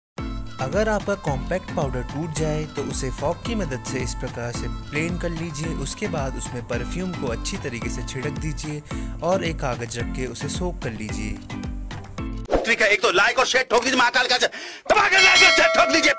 How to fix compact powder !!?? 😲
In this video you will find how to fix compact powder at home